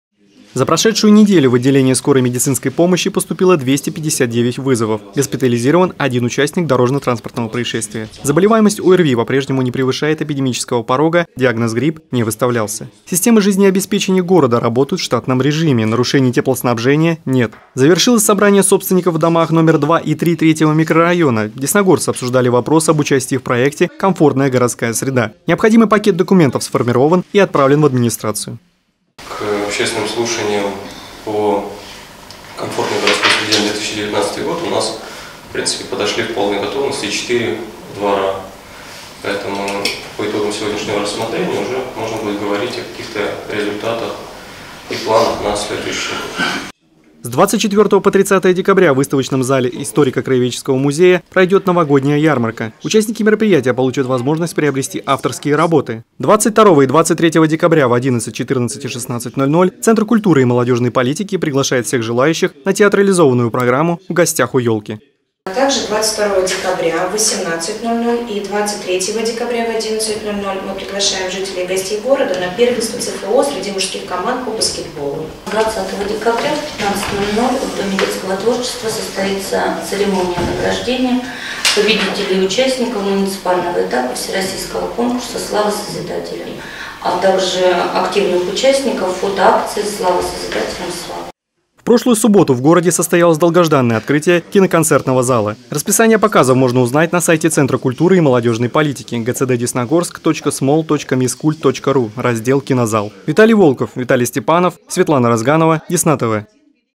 Десна-ТВ: Руководители подразделений обсудили текущую обстановку в городе
Работа медсанчасти, задачи коммунальных служб, культурные мероприятия в городе. Эти и другие актуальные вопросы обсудили руководители подразделений на еженедельном совещании с главой города.